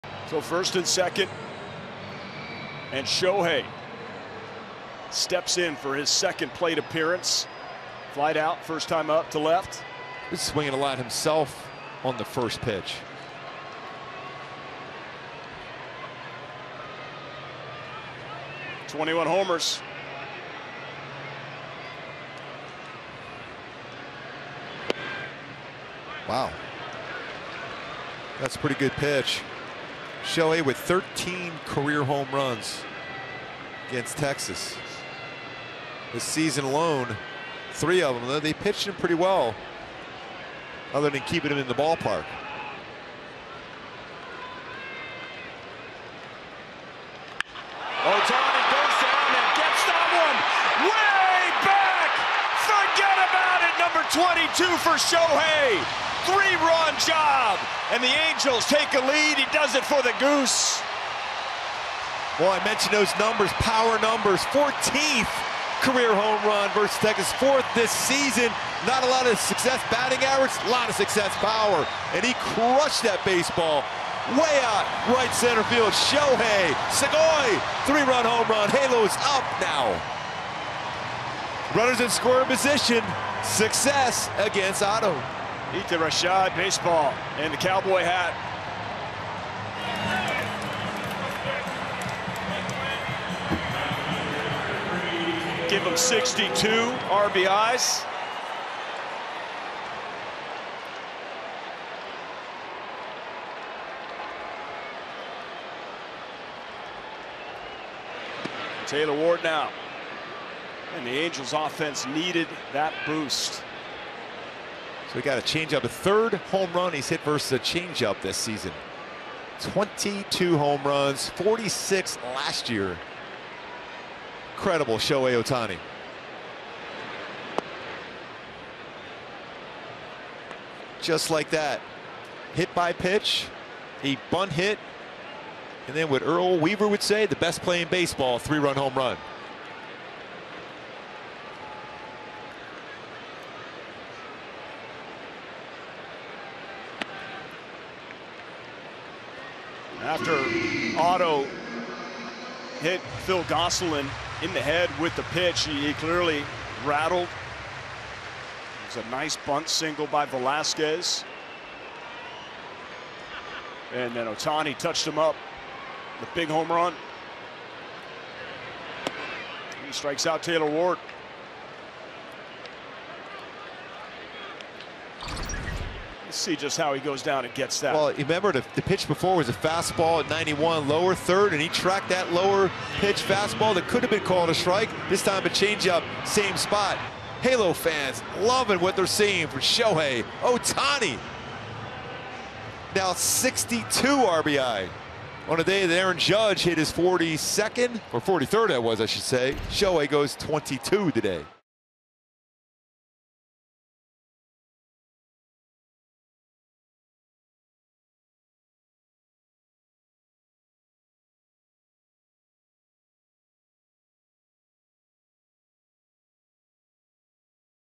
Shohei Ohtani with a MONSTER home run for the lead!
Watch it fly, Ohtani.